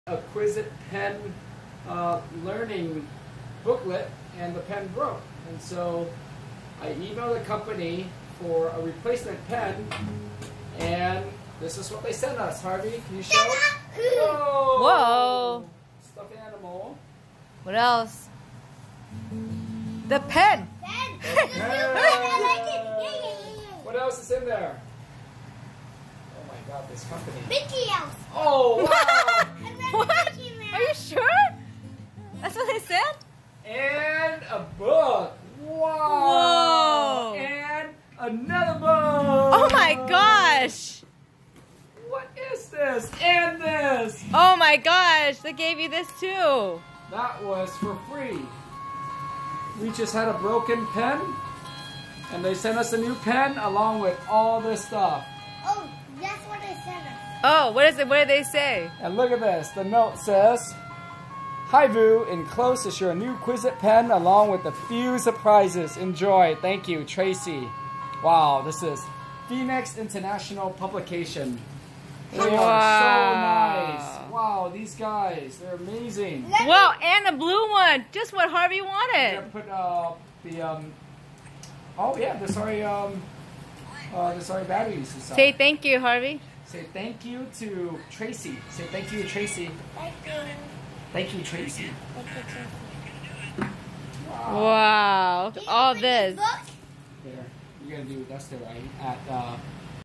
Quiz it pen replacement and surprise presents.
Harvey (5). His quiz pen broke and they sent him this packet of goodies!!!